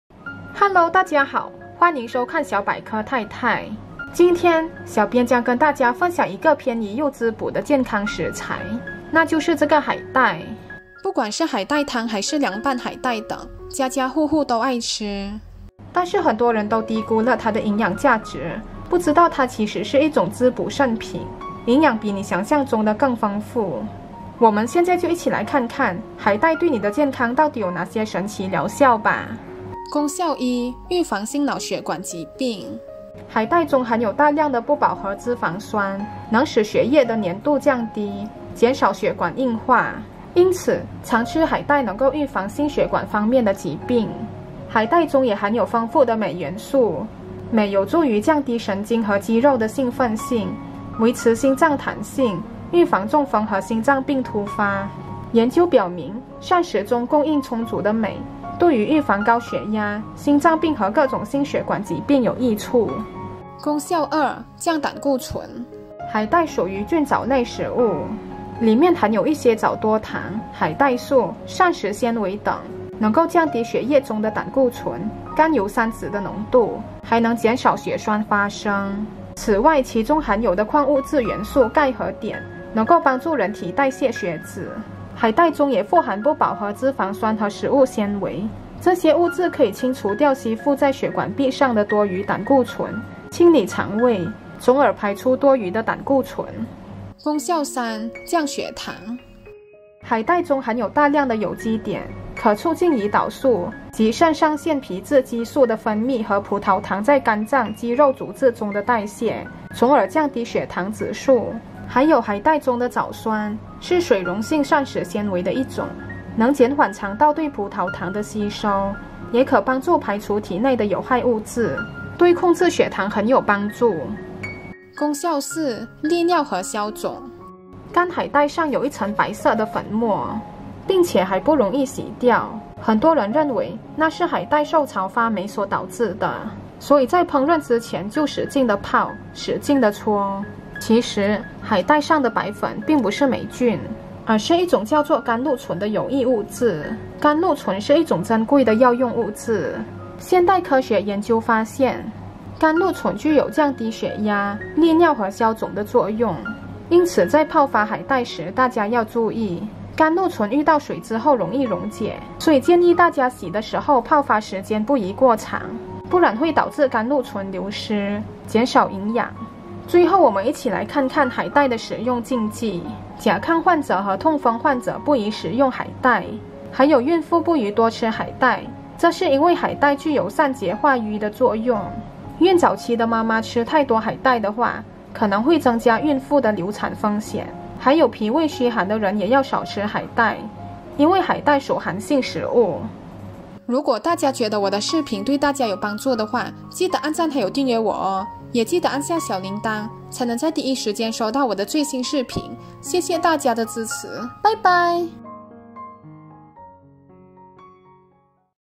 它是天然的降三高藥！每周吃2次，化血栓清血管，血糖乖乖降下來！你一定要試試！
Hello 大家好！歡迎收看小百科太太。今天，小編將跟大家分享一個便宜又滋補的健康食材，那就是這個海帶。不管是海帶湯還是涼拌海帶等，家家戶戶都愛吃。但是很多人都低估了它的營養價值，不知道它其實是一種滋補聖品，營養比你想象中的更豐富。我們現在就一起來看看海帶對你的健康到底有哪些神奇療效吧!